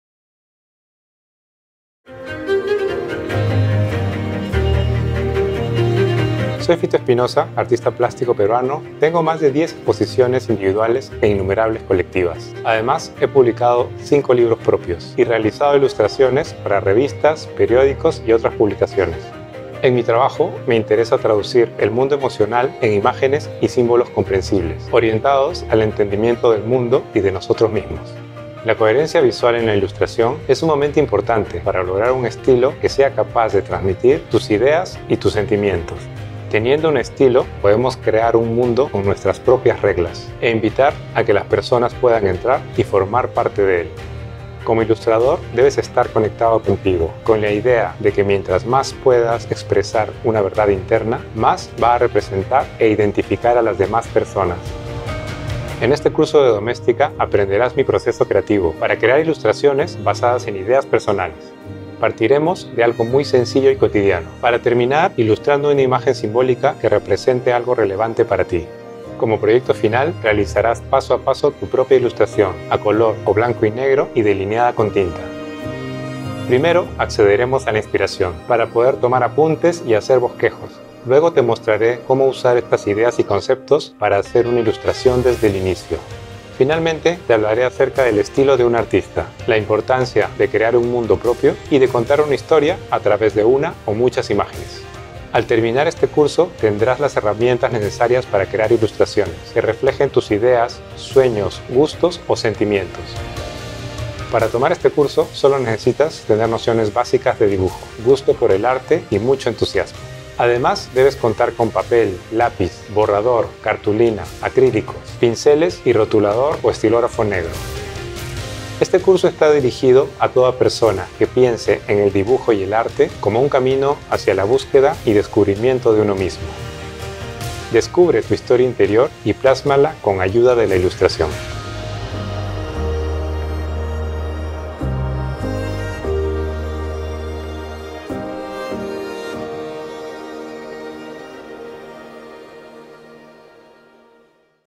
Ilustración Artística: Dibuja tu Imaginario | Un curso de Fito Espinosa | Domestika
Conecta con tus emociones y aprende a plasmarlas en imágenes simbólicas y personales

La ilustración artística es una poderosa herramienta que te permite explorar tu yo interior y expresarlo con el mundo mediante imágenes. El pintor, ilustrador y artista plástico peruano, Fito Espinosa, ahonda en la mente y las emociones para traducirlas en imágenes simbólicas comprensibles, orientadas al entendimiento del mundo y de uno sí mismo.

En este curso Fito Espinosa te enseñará cómo ilustrar desde la conexión con tus emociones e ideas más personales. Te explicará paso a paso cómo empezar desde una pequeña idea o intuición a modo de creación poética, para pasar a crear bocetos y desarrollar imágenes que expresen tu imaginario y tu especial punto de vista.